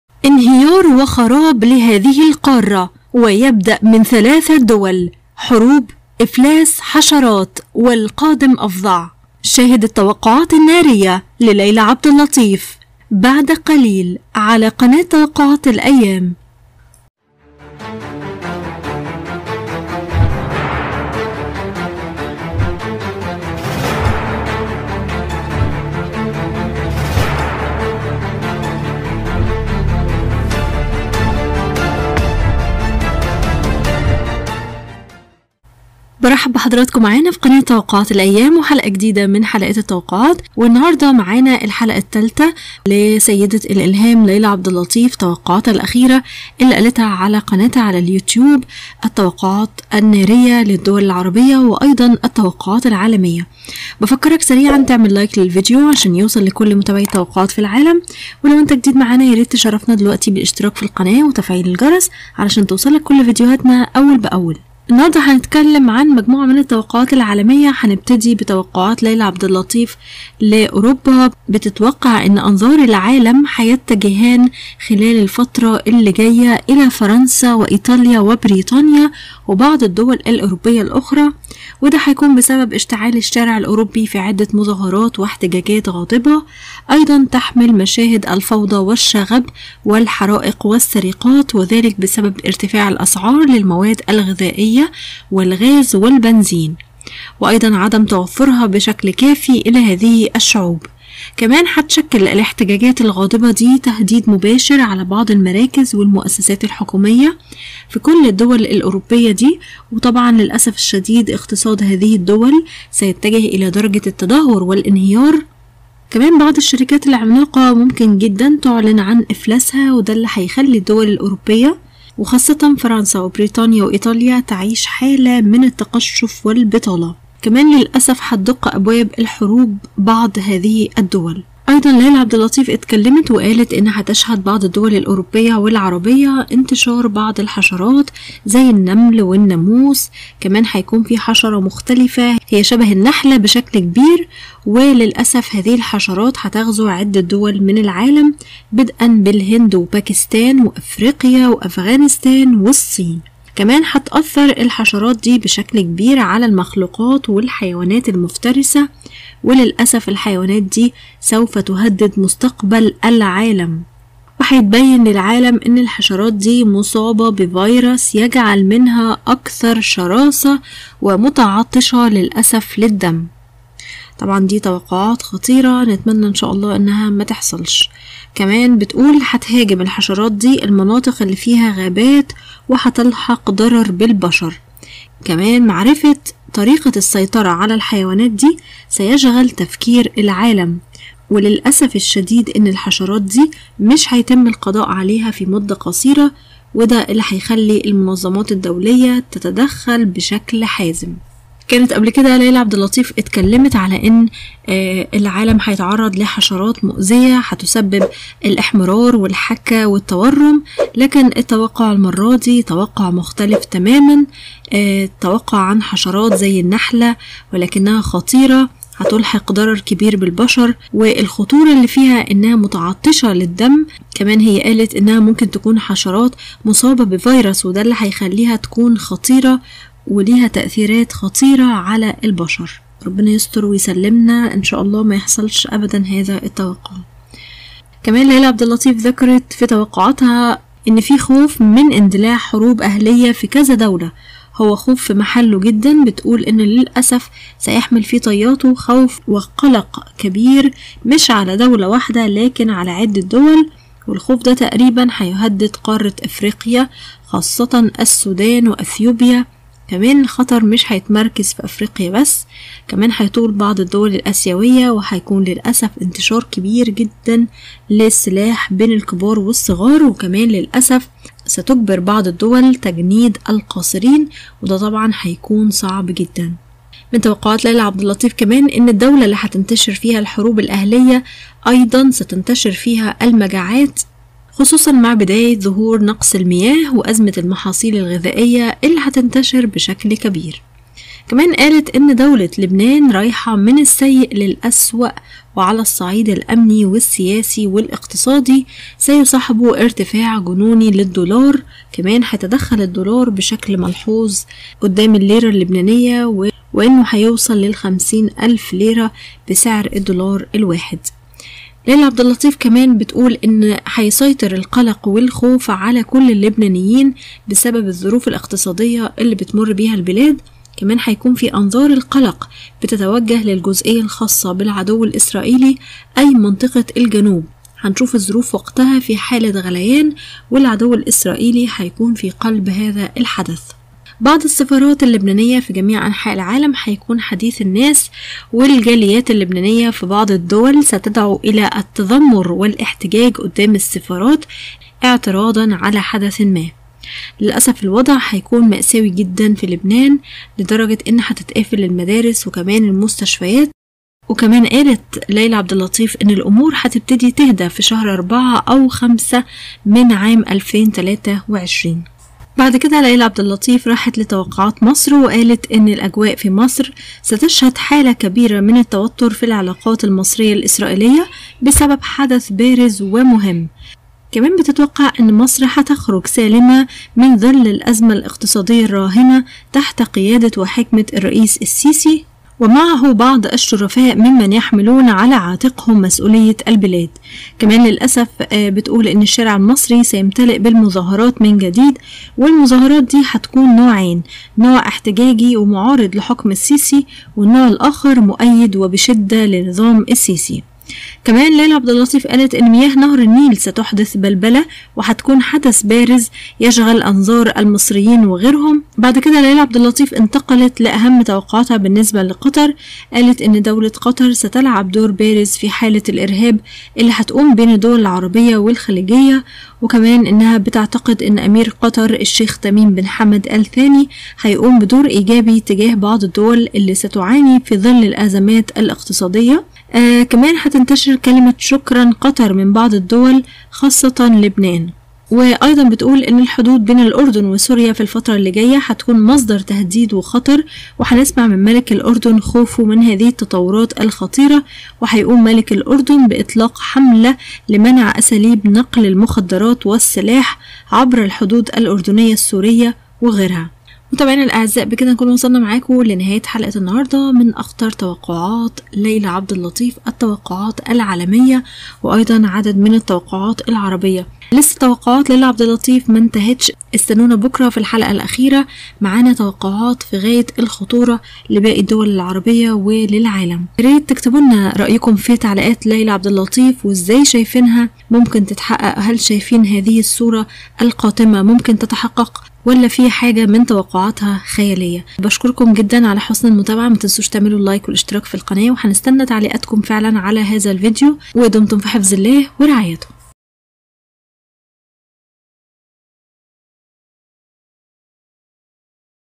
أخطر ما توقعت ليلي عبد اللطيف انهيار وخراب لهذه القارة ويبدأ من ٣ دول .حروب افلاس حشرات والقادم افظع
أخطر ما توقعت ليلي عبد اللطيف انهيار وخراب لهذه القارة ويبدأ من ٣ دول .حروب افلاس حشرات والقادم افظع توقعات 2023